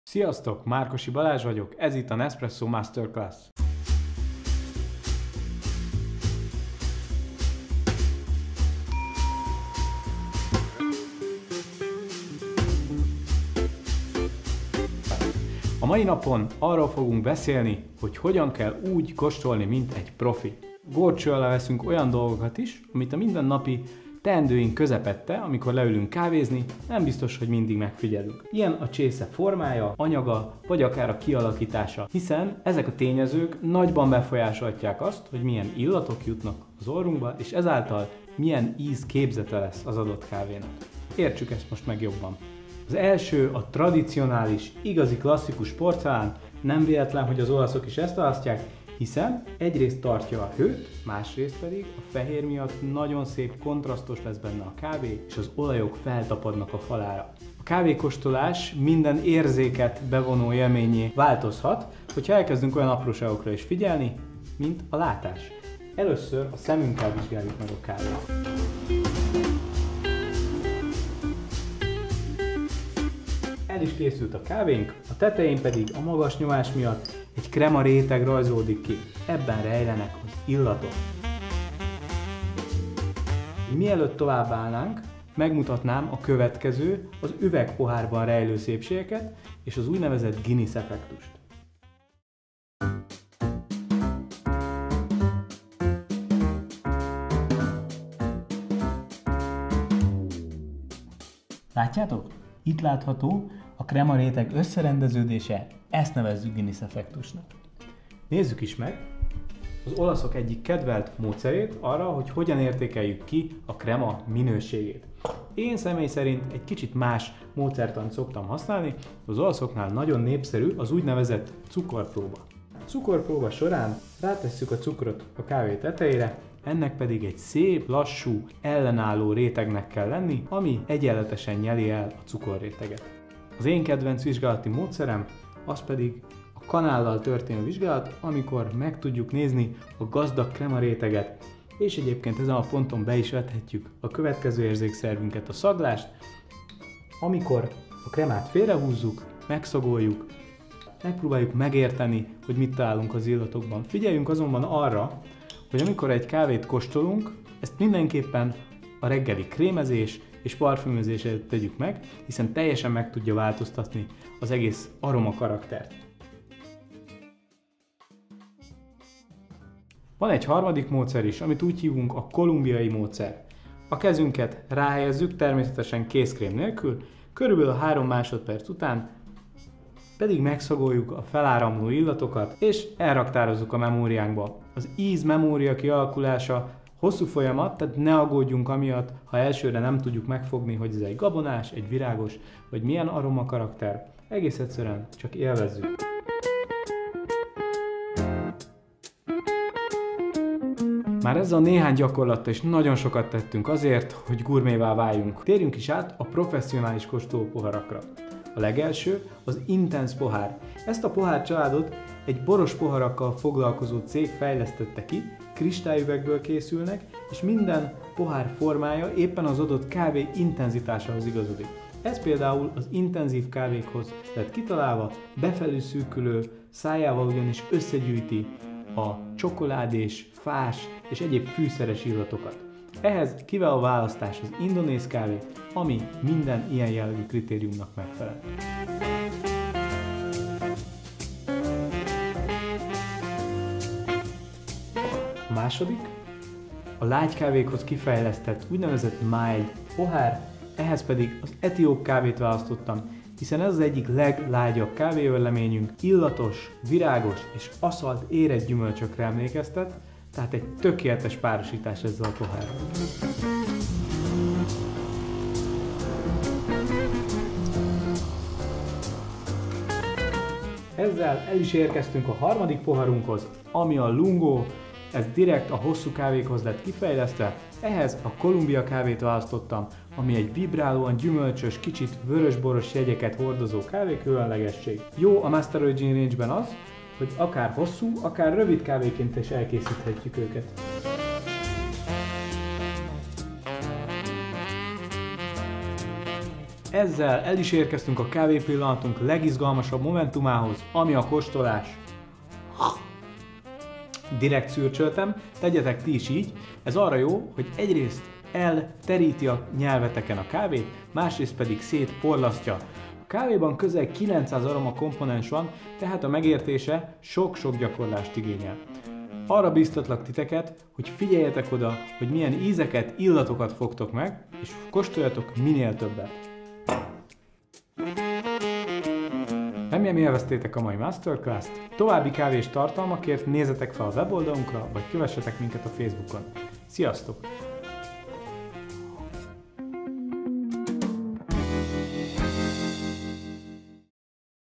Nespresso Masterclass - Hogyan kóstoljunk úgy, mint egy profi | HU
Gondolkozott már, hogy miért éppen porcelán csészét használnak az olaszok? És azon, hogyan értékelje az Ön előtt gőzölgő kávé minőségét, ízét vagy aromvilágát? Ebben  a videóban bemutatjuk, hogyan kóstolhatja a kávéját úgy, mint egy szakértő. Az aromacsaládok jellegzetességei mellett, hasznos tippeket adunk a különböző csészetípusok előnyeire vonatkozóan is – az élmény szempontjából ugyanis nem mindegy, hogy gyümölcsös/virágos, kiegyensúlyozott vagy intenzív kávénkhoz milyen csészét választunk - , és ami a legfontosabb, ez a legjobb módja, hogy mélyebben megértsük kávéérzékelés élményét.